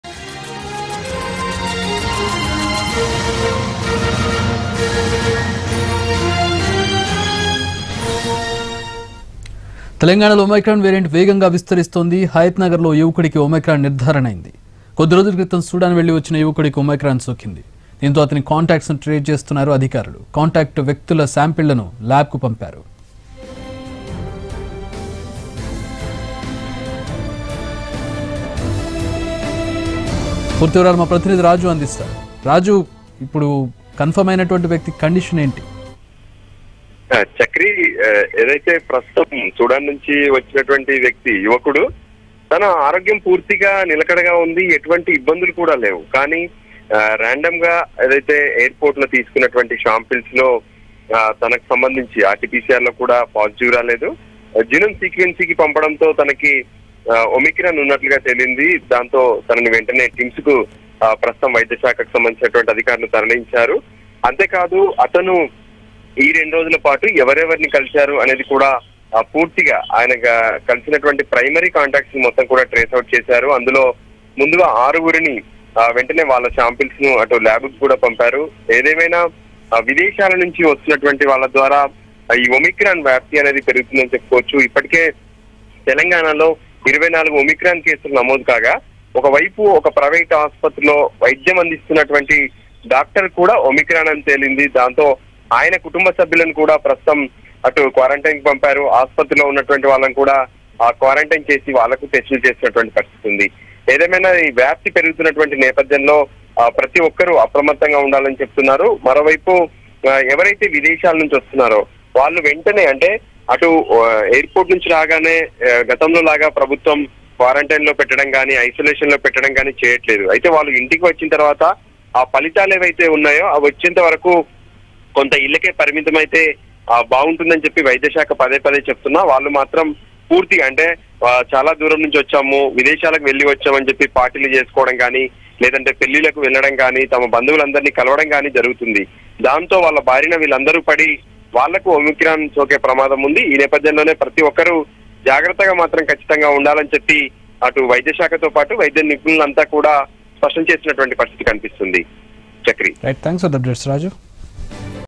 Omicron Threat In Hyderabad, 1 More Case Detected In Hayathnagar | Ntv
Omicron Threat In Hyderabad, 1 More Cases Detected In Hayathnagar